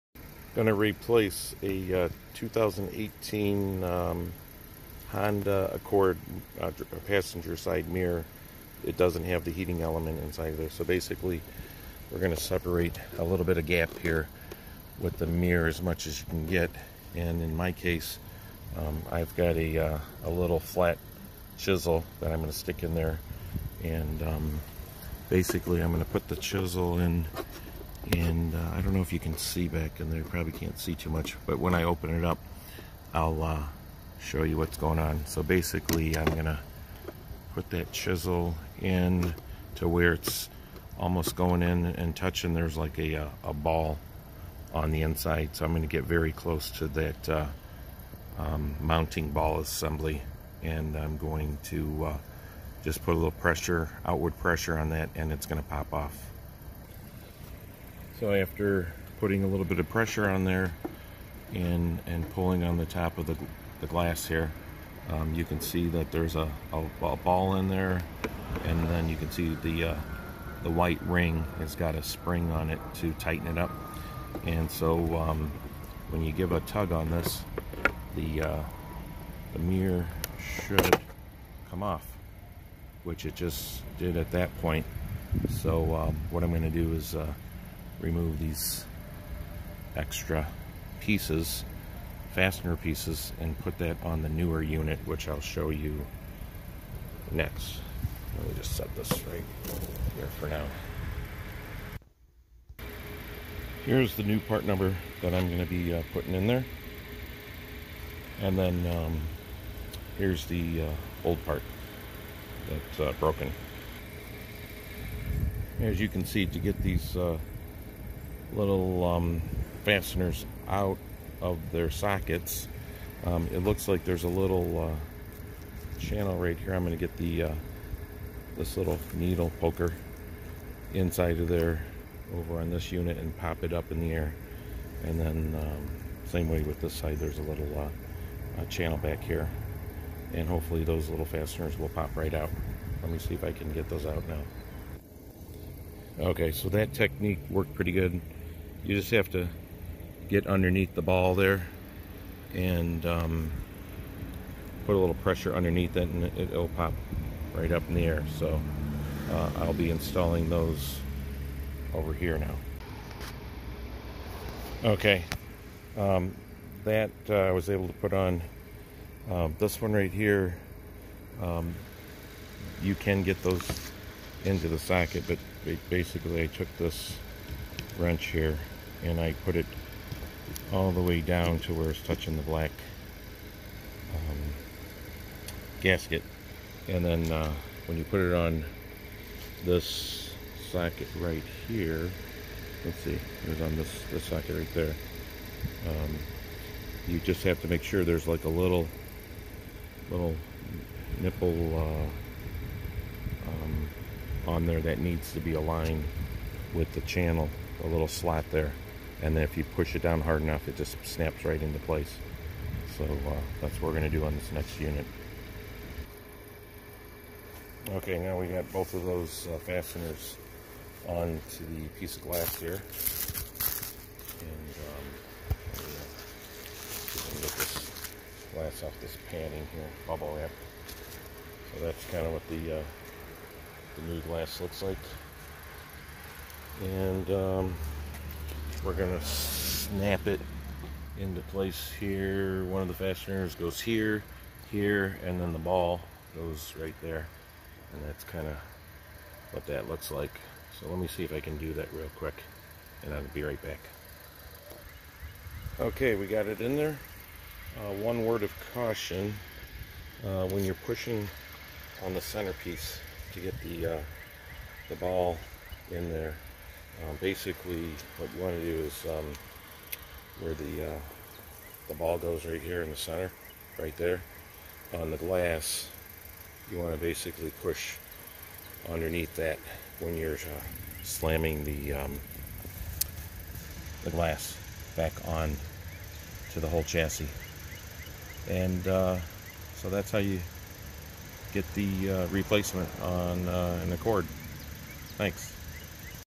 2018 Honda Accord Passenger Side Mirror Replacement
The video shows the steps necessary to replace the mirror within a 5 minute period. My mirror wasn't a heated one so the replacement was a little easier to do.

Part # replaced was 76203-TVA-A01 (Mirror Sub Assy Right)